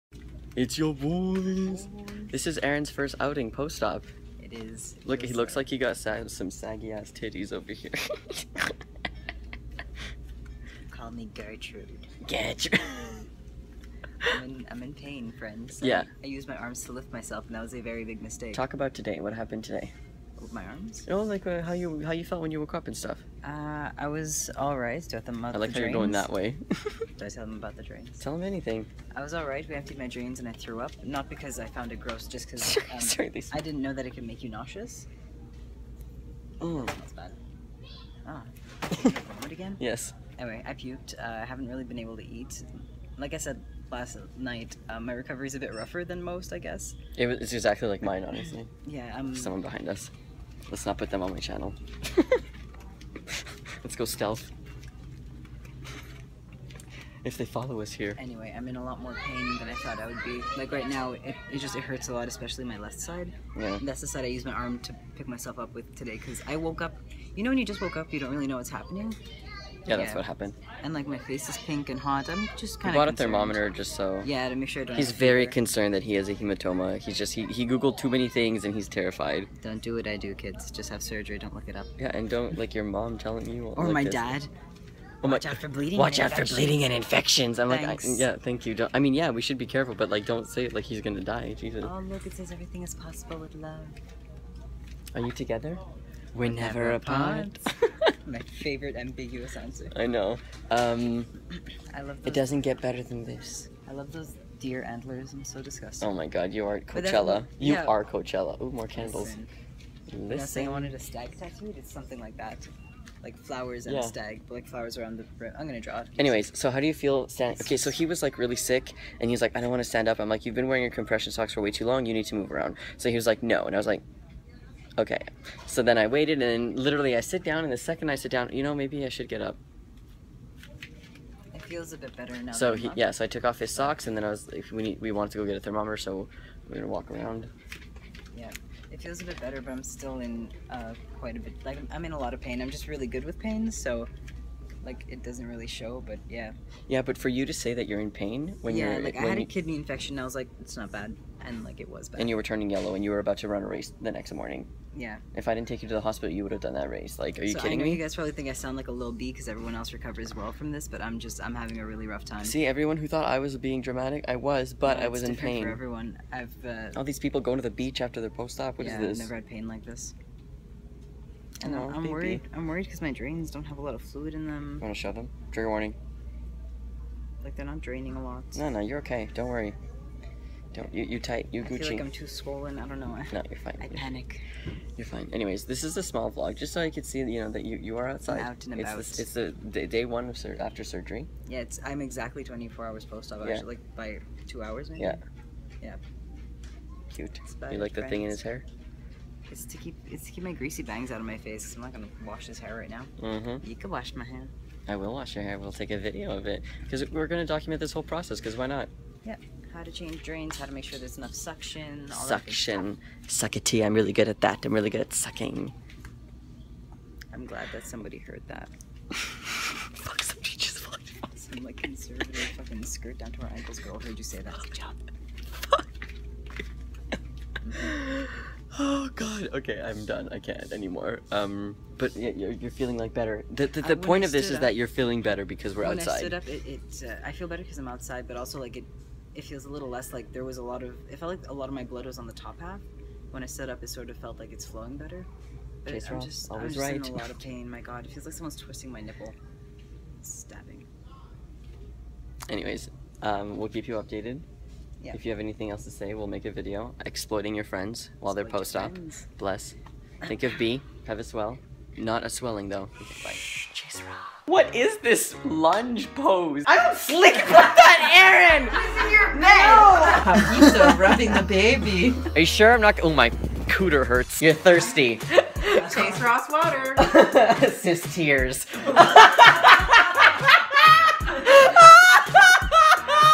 CHASE RAW // VLOG #23 // AARONS FIRST TIME OUTSIDE POST-OP!
Hey! Aaron and I go to target and it's so exciting l o l

[[CC SOON]]

Facebook.com/uppercasechase1
Instagram.com/uppercasechase